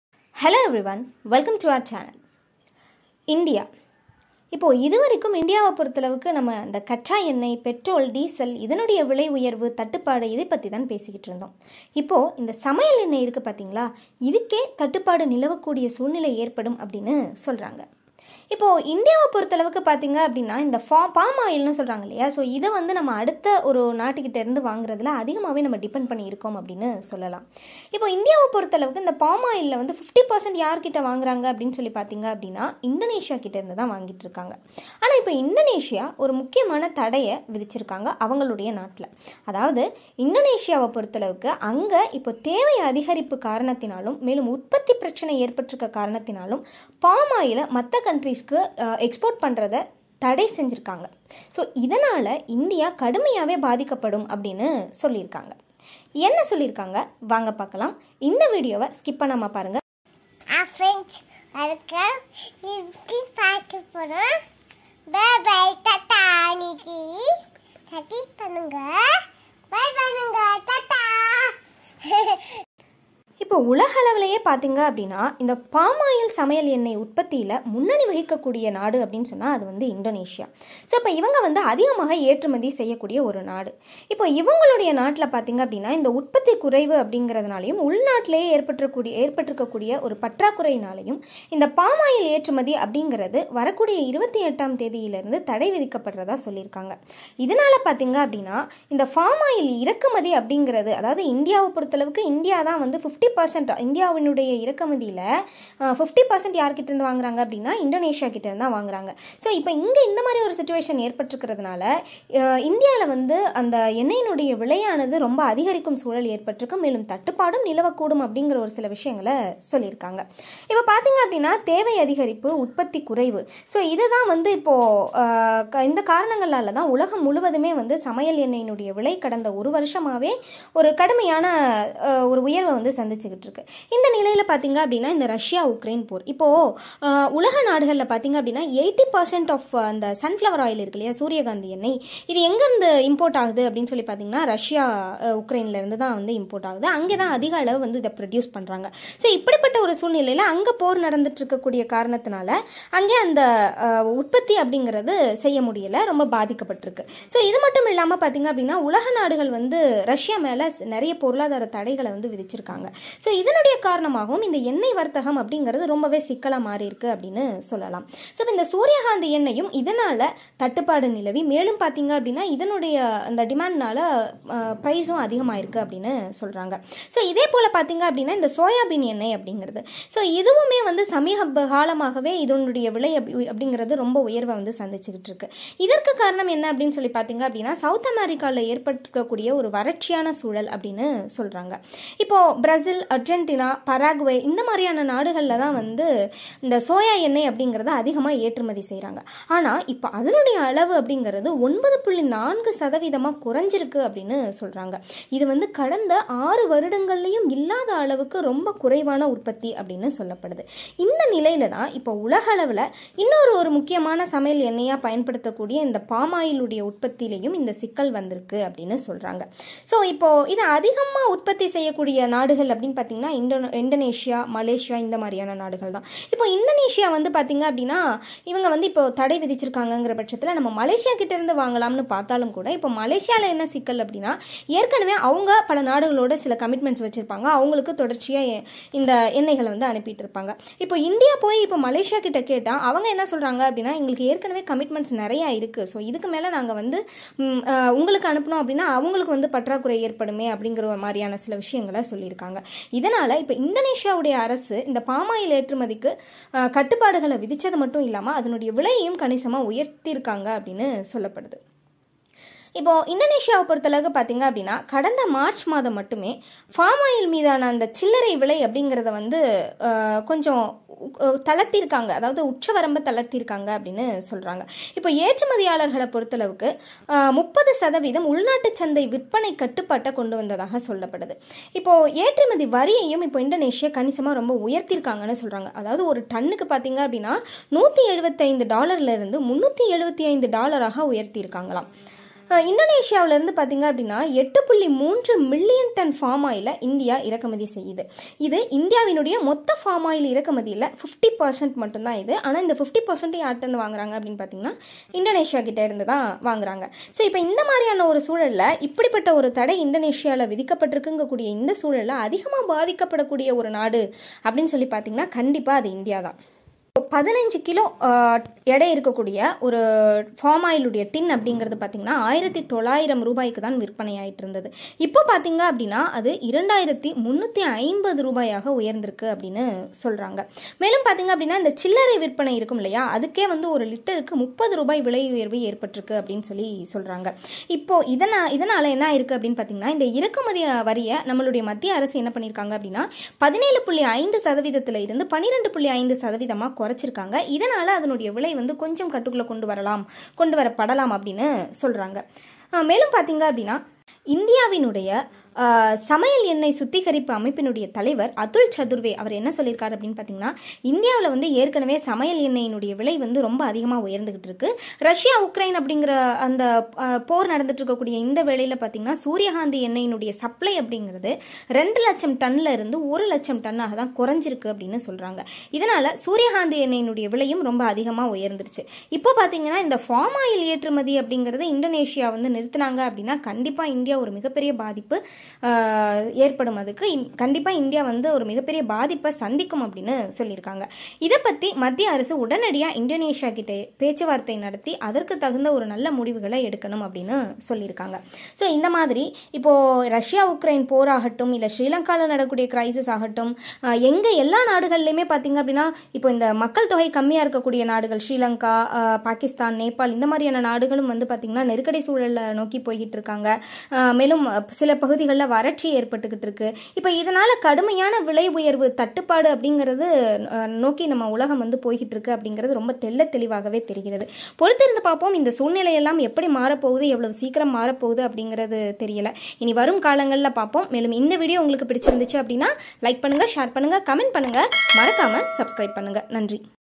How Much Should India Worry Over Indonesian Plam Oil Export | Tamil | Rajesh Karmegam
This video explains the ban of palm oil export in Indonesia. India depends on 50% of Palm oil of total imports from Indonesia . This ban of palm oil may affect India .